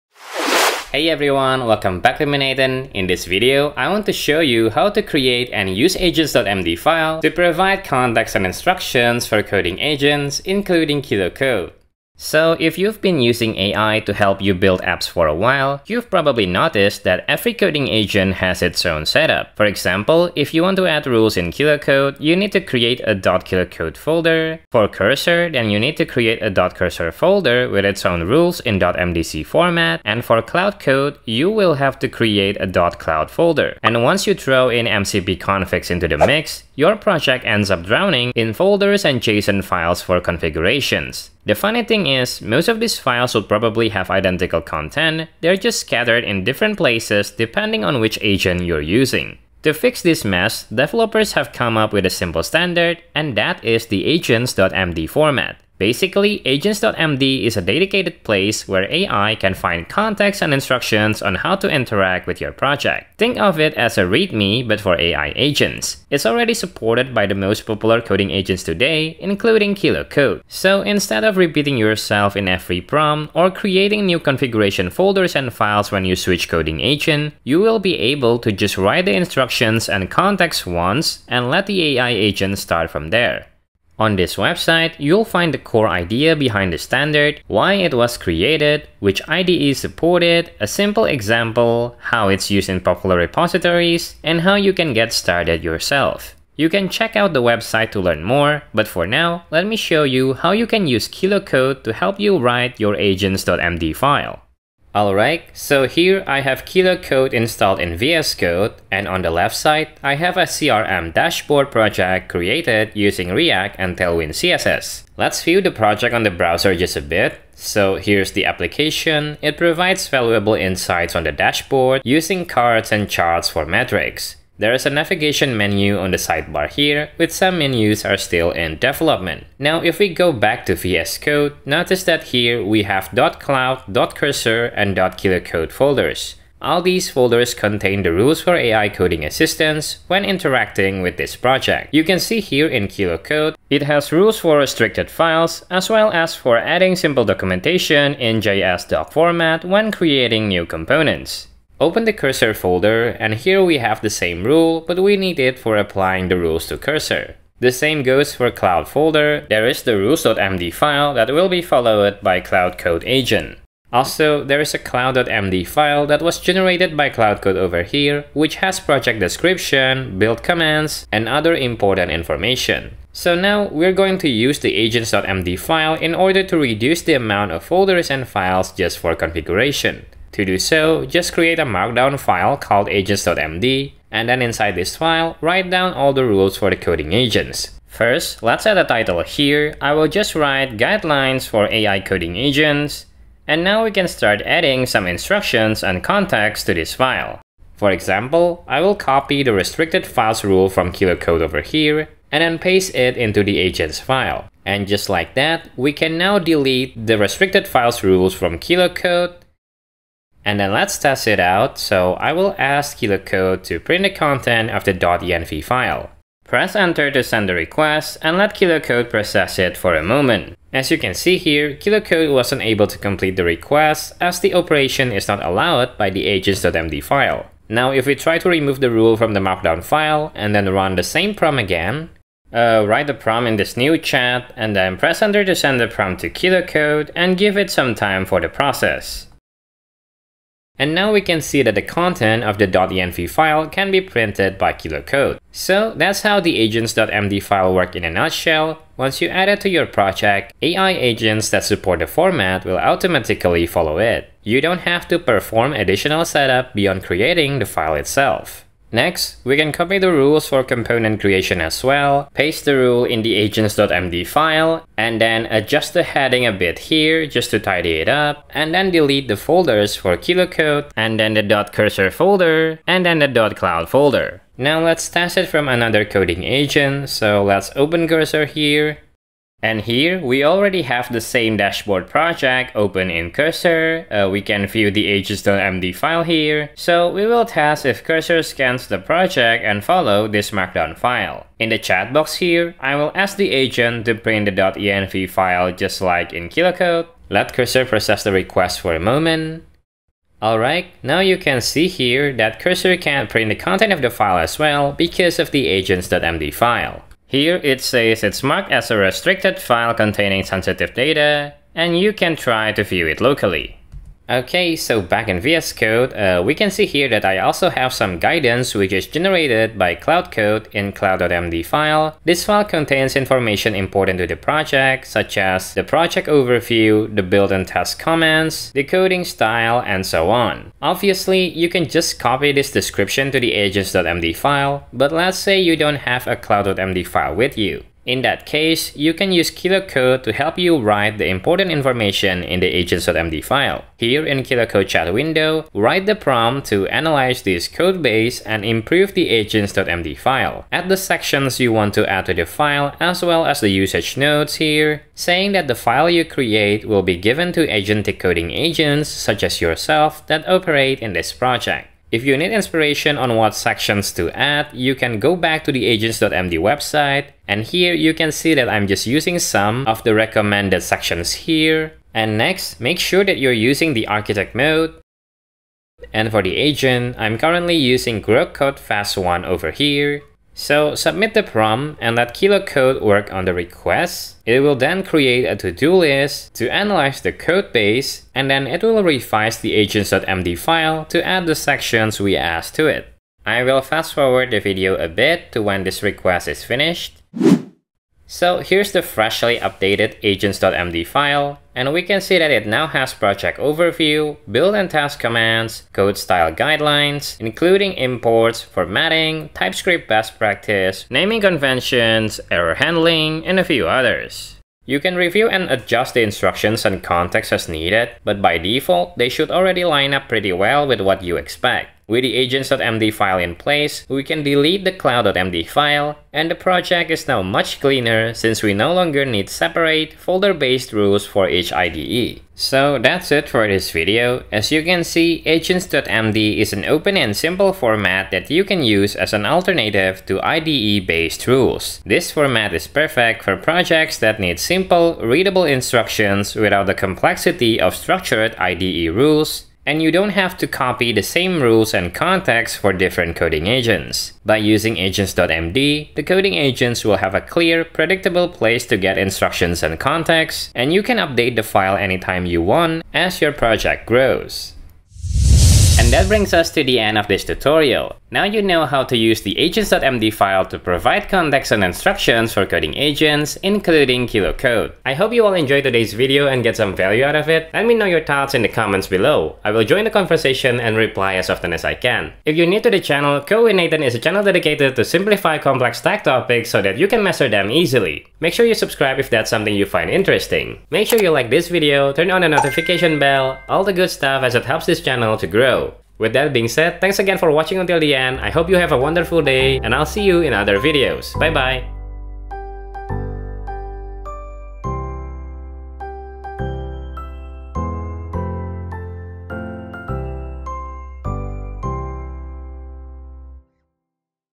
How to Use AGENTS.md File (Kilo Code) 99% of AI Tools Will Use It Now
In this video, I’ll show you why AGENTS.md is becoming a game-changer for AI tools, and walk you through exactly how to get started with it today. Don’t miss this one.